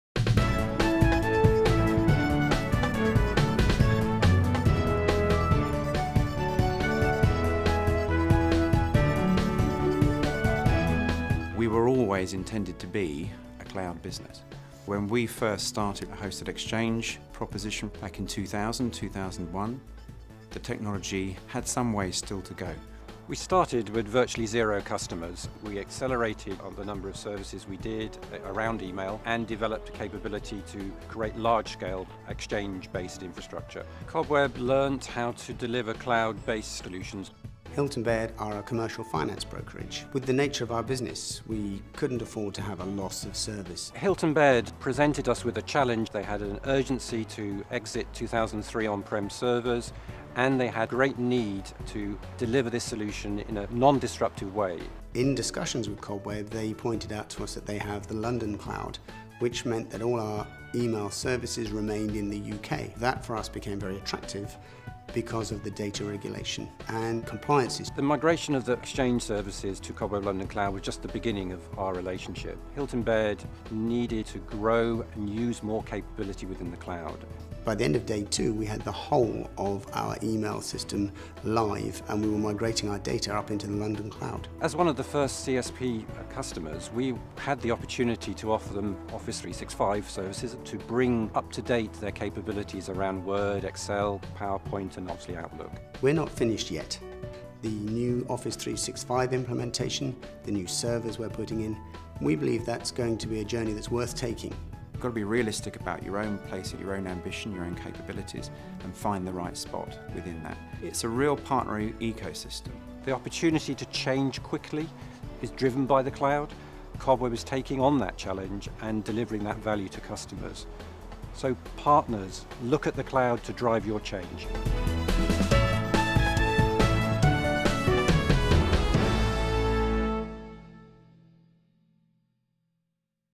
Microsoft Case Study - The #1 UK Cloud Solution Provider showcases its hybrid cloud solutions
Cobweb offering hybrid cloud to customer needing Hosted Exchange and Office 365 solutions